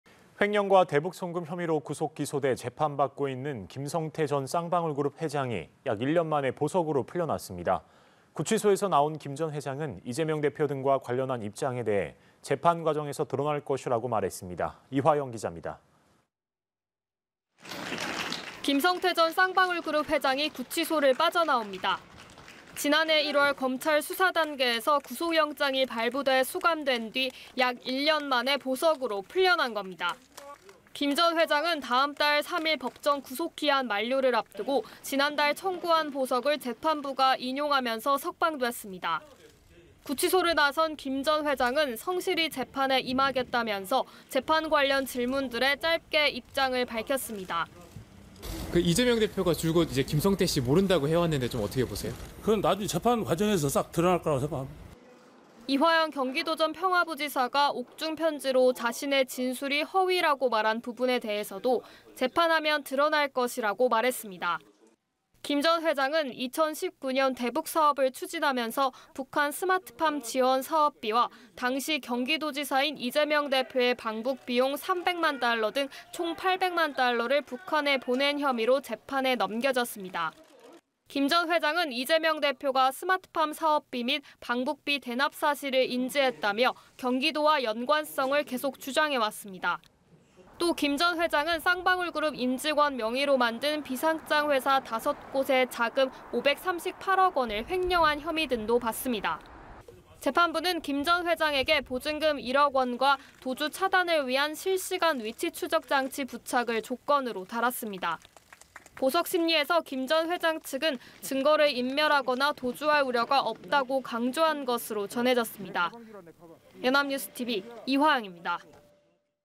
대북송금 혐의 김성태 보석 석방…"재판서 드러날 것" / 연합뉴스TV (YonhapnewsTV)
대북송금 혐의 김성태 보석 석방…"재판서 드러날 것"

[앵커]

횡령과 대북 송금 혐의로 구속 기소돼 재판받고 있는 김성태 전 쌍방울그룹 회장이 약 1년 만에 보석으로 풀려났습니다.

구치소에서 나온 김 전 회장은 이재명 대표 등과 관련한 입장에 대해 "재판 과정에서 드러날 것"이라고 말했습니다.

이화영 기자입니다.

[기자]

김성태 전 쌍방울 그룹 회장이 구치소를 빠져나옵니다.

지난해 1월 검찰 수사 단계에서 구속영장이 발부돼 수감된 뒤 약 1년 만에 보석으로 풀려난 겁니다.

김 전 회장은 다음달 3일 법정 구속 기한 만료를 앞두고 지난달 청구한 보석을 재판부가 인용하면서 석방됐습니다.

구치소를 나선 김 전 회장은 "성실히 재판에 임하겠다"면서 재판 관련 질문들에 짧게 입장을 밝혔습니다.

[김성태/전 쌍방울 그룹 회장] "(이재명 대표가 김성태 씨 모른다고 해왔는데 어떻게 보세요?) 그건 나중에 재판 과정에서 싹 드러날 것이라고 생각합니다."

이화영 경기도 전 평화부지사가 옥중편지로 자신의 진술이 허위라고 말한 부분에 대해서도 "재판하면 드러날 것"이라고 말했습니다.

김 전 회장은 2019년 대북 사업을 추진하면서 북한 스마트팜 지원 사업비와 당시 경기도지사인 이재명 대표의 방북 비용 300만 달러 등 총 800만 달러를 북한에 보낸 혐의로 재판에 넘겨졌습니다.

김 전 회장은 이재명 대표가 스마트팜 사업비 및 방북비 대납 사실을 인지했다며 경기도와 연관성을 계속 주장해 왔습니다.

또 김 전 회장은 쌍방울 그룹 임직원 명의로 만든 비상장 회사 5곳의 자금 538억 원을 횡령한 혐의 등도 받습니다.

재판부는 김 전 회장에게 보증금 1억원과 도주 차단을 위한 실시간 위치추적 장치 부착을 조건으로 달았습니다.

보석 심리에서 김 전 회장 측은 증거를 인멸하거나 도주할 우려가 없다고 강조한 것으로 전해졌습니다.

연합뉴스TV 이화영입니다.

(끝)